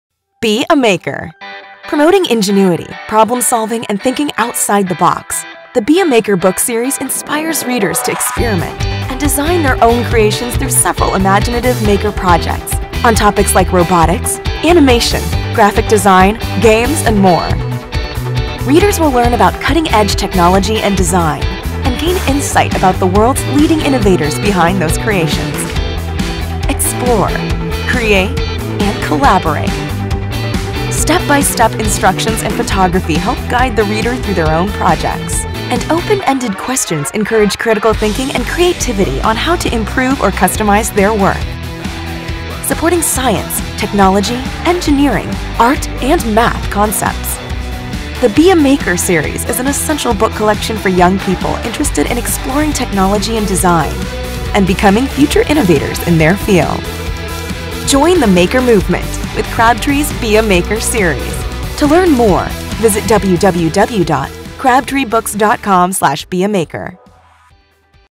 Be A Maker!
The Maker movement celebrates qualities such as ingenuity, problem solving, collaboration, and thinking “outside the box.” The Be a Maker! series embraces these same ideas to inspire new “makers” to explore and experiment with the world around them. Supporting STEAM initiatives and 21st Century skills, each title in this timely series focuses on a field or area of interest, from robotics, graphic design, and animation to game design, music, and fashion. Readers discover how “makers” in the field use their skills, imaginations, and fearless, pioneering attitudes to make breakthrough discoveries and inventions. Each book features several maker projects with step-by-step instructions and photography that guide readers through their own projects. Open-ended critical thinking questions encourage readers to consider how they might improve or customize their work. Be a Maker! inspires young people to explore their passions and make their mark in the world!